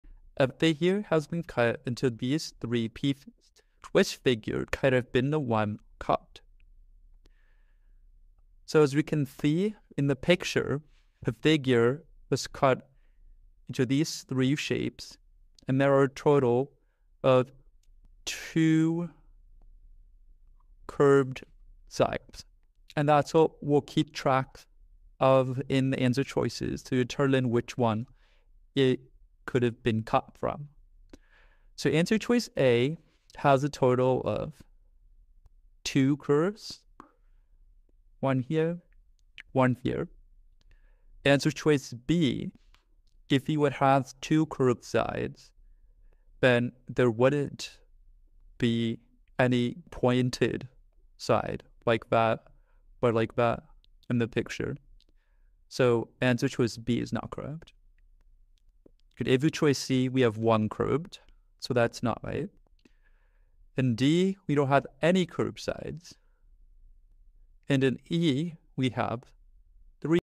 Elementary Logic Puzzle | Which Shape Was Cut? MUST KNOW
puzzle comes from the 2019 Math Kangaroo (Levels 1–2).
A figure was cut into three pieces — which original shape could it be?
The trick is to observe the curved sides. The pieces together show two curves, so only option A fits.
A simple but fun logic puzzle for elementary students.